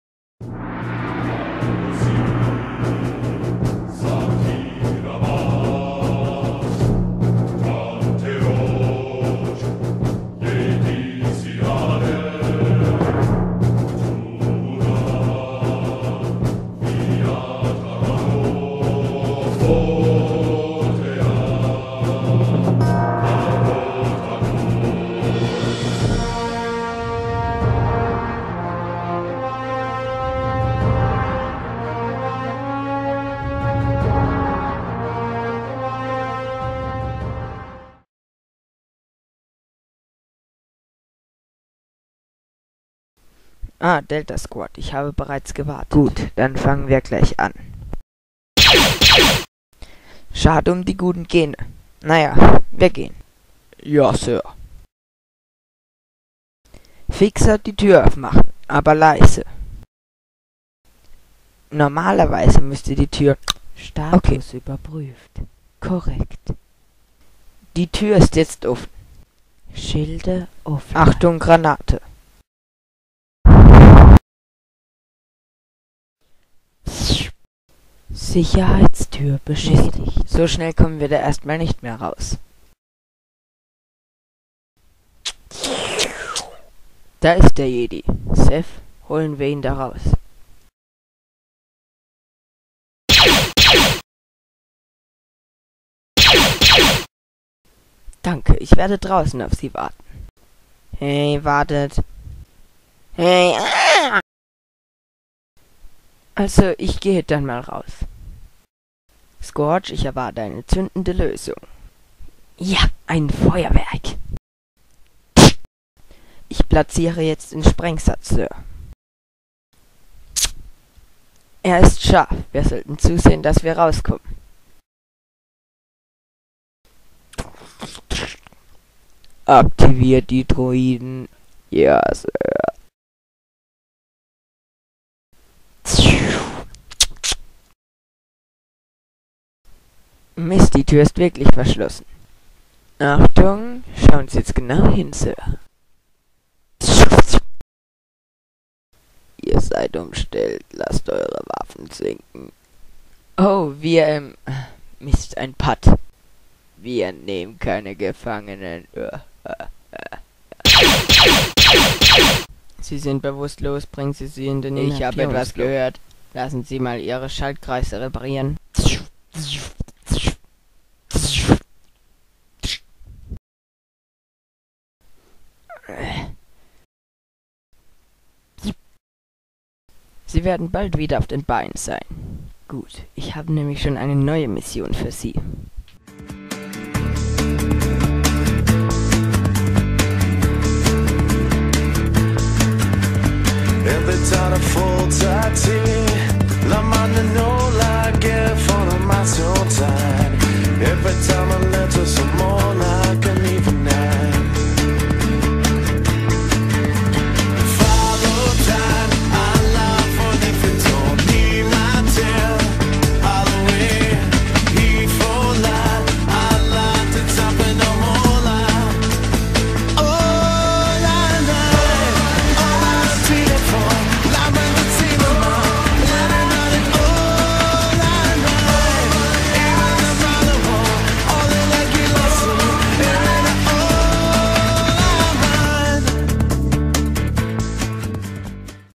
Star wars Empires I
Der erste Film der Serie lego Star Wars Empires. Der Film ist merh ein Test als ein richtiger Film um zu sehen ob es sich lohnt noch mehr davon zu machen. Also bitte Bewertung abgeben. Danke.